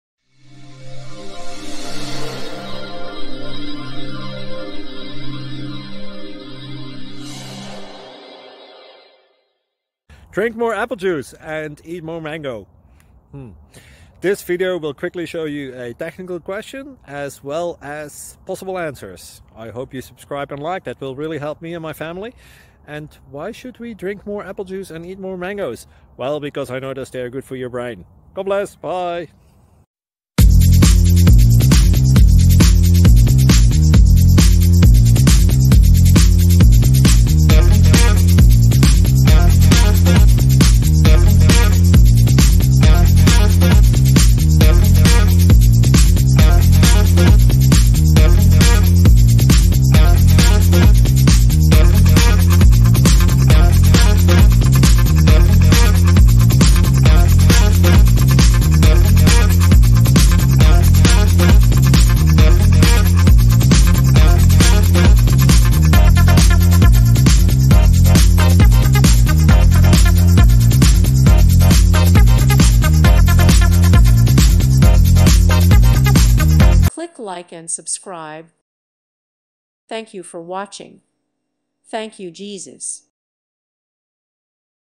Breaking apart a large javascript application that uses the object namespacing pattern
Software Engineering: Breaking apart a large javascript application that uses the object namespacing pattern

With thanks & praise to God, and with thanks to the many people who have made this project possible! You are responsible for your own actions.